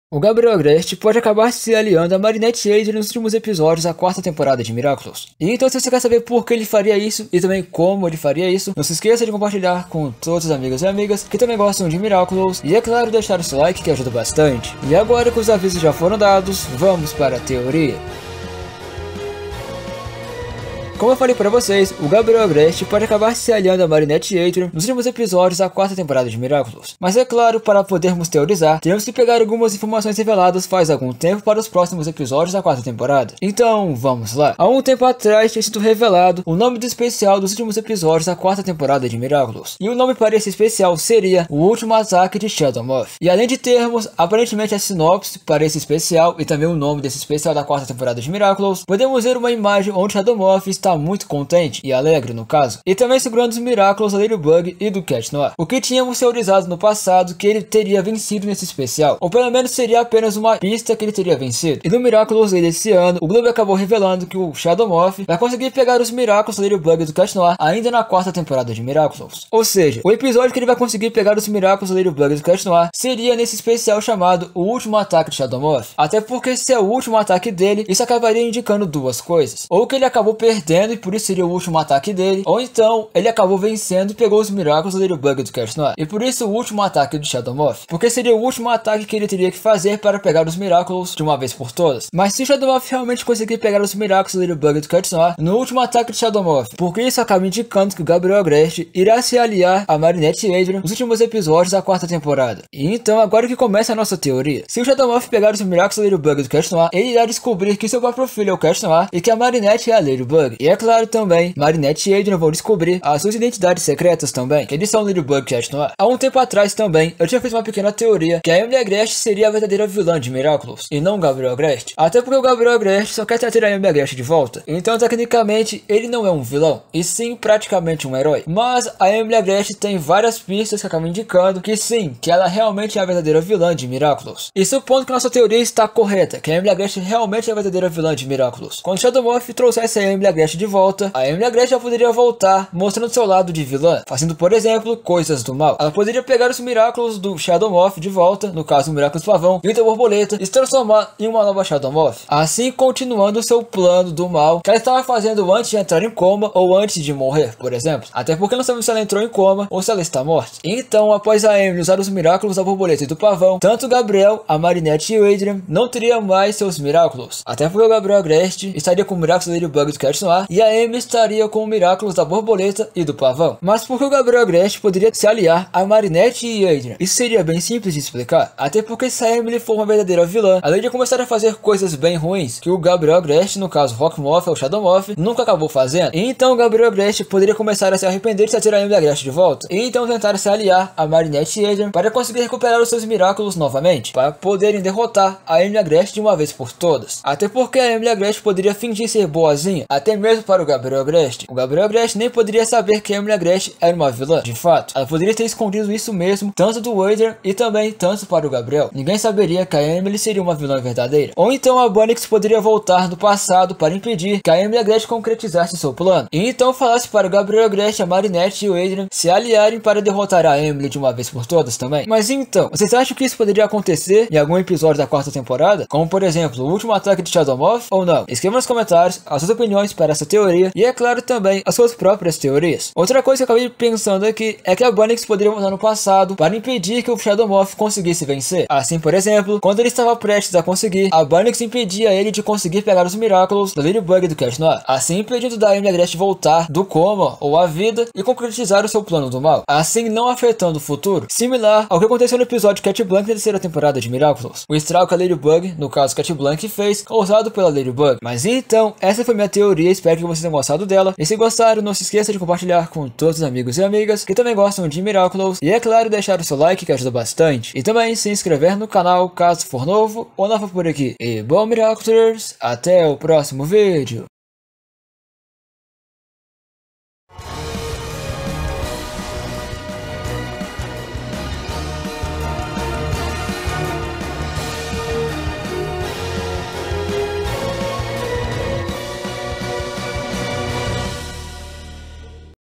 GABRIEL VAI SE ALIAR AOS HERÓIS PARA DERROTAR A EMILIE???
Curtiu a capa do vídeo? Meu amigo fez ela para mim! Se puderem, sigam ele no Instagram:

Vídeo: GABRIEL VAI SE ALIAR AOS HERÓIS PARA DERROTAR A EMILIE???
Resumo do vídeo: Em um dos episódios da quarta temporada, o Gabriel vai pegar os Miraculous da Ladybug e do Cat Noir, e talvez, trazer finalmente a Emilie Agreste de volta! Mas ela pode ser uma vilã, e pegar os Miraculous do Gabriel Agreste, e os Miraculous do Gato Preto e da Joaninha, para ter o poder absoluto, e concretizar seu possível plano do mal! Então o Gabriel, pode se aliar aos seus inimigos, no caso Marinette e Adrien, que são Ladybug e Cat Noir, para derrotar a Emilie Agreste, que mostrou ser uma vilã em segredo! Quer saber se isso pode ser possível ou não? Então assista à minha teoria!